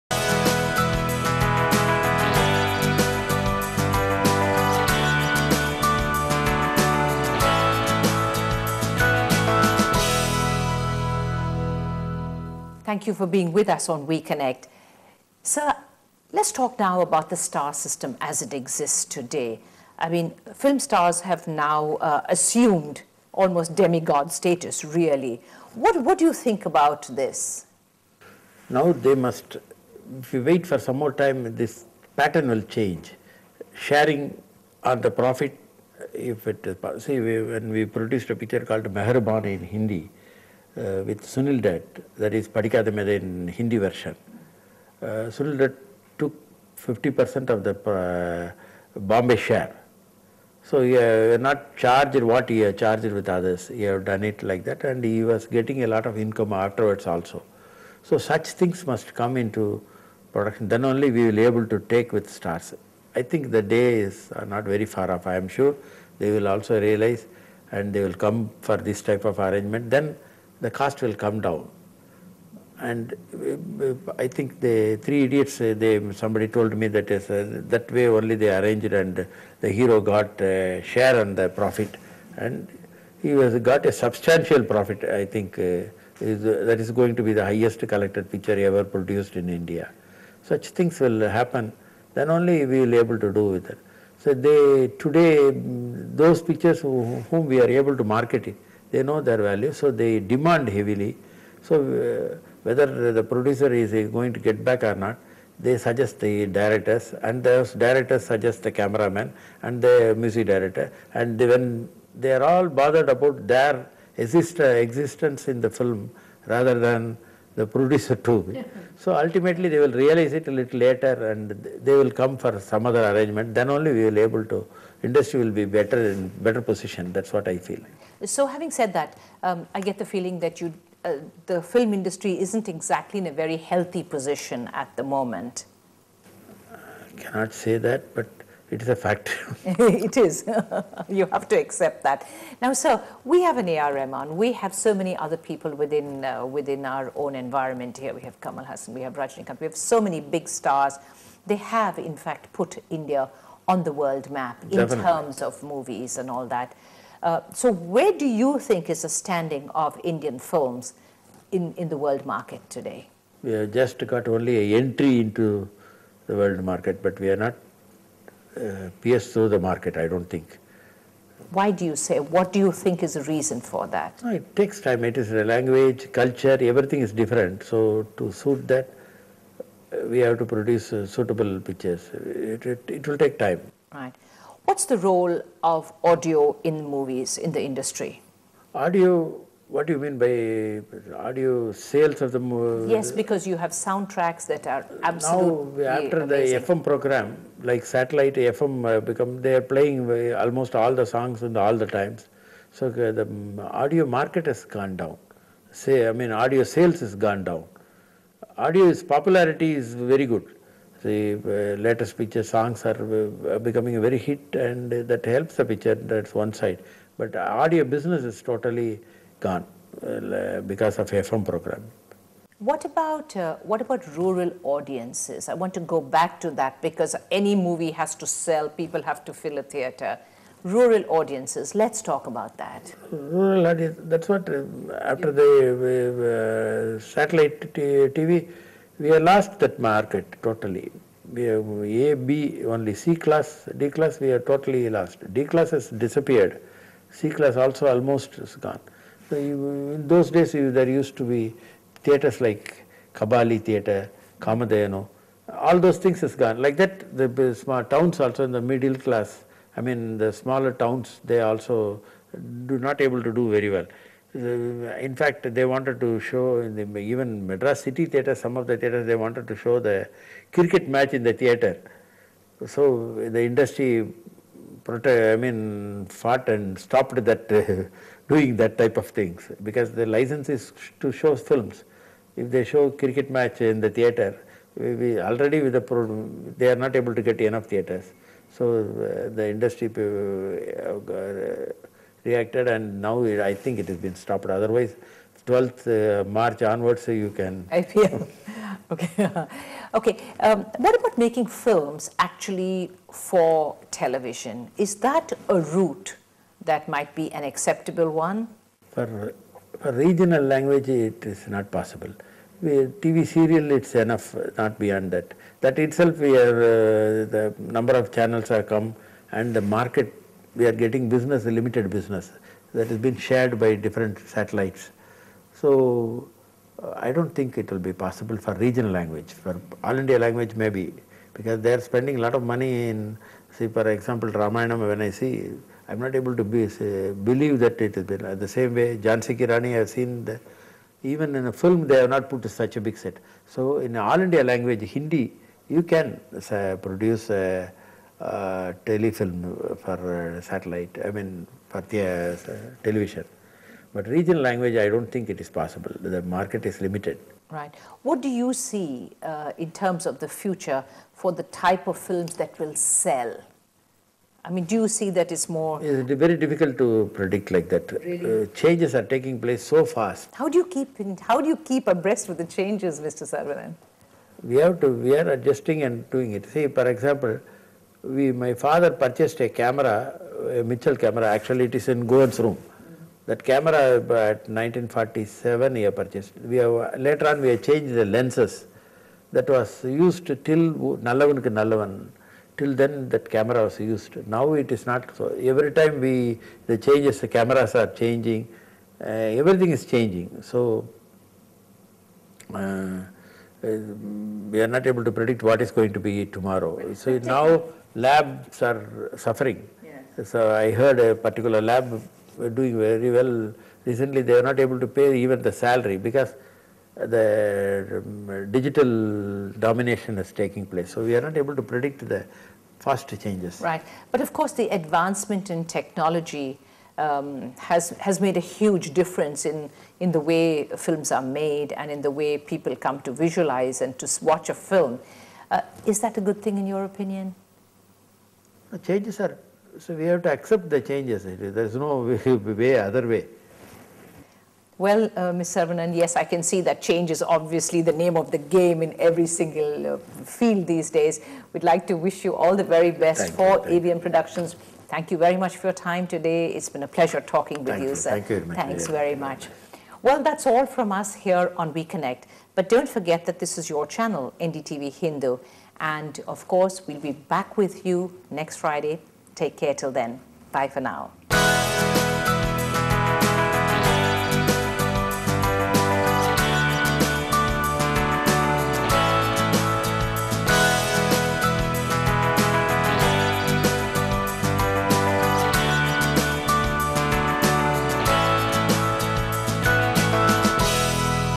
We Connect - AVM Saravanan - Seg 3
In a wide ranging interview AVM Saravanan,  of AVM Productions, traces the history of the country's most prolific film production house.  Talking to Anchor Jennifer Arul,  the normally reticient film maker opened up about the current state of the industry, the star system and the competition that Television poses for the movie-making business.